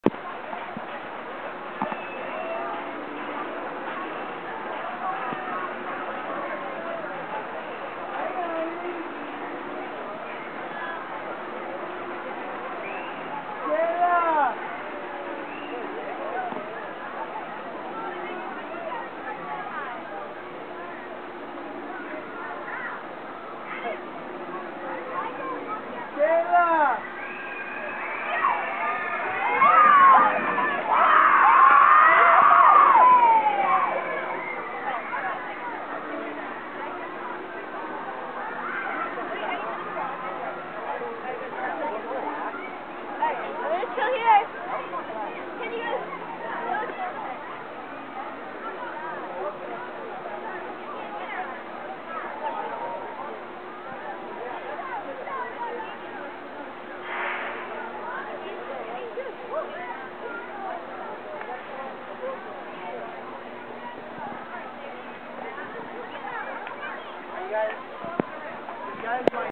Boston Harbor Patrol - off ride
This is the Boston Harbor Patrol ride at Canobie Lake Park in Salem, NH.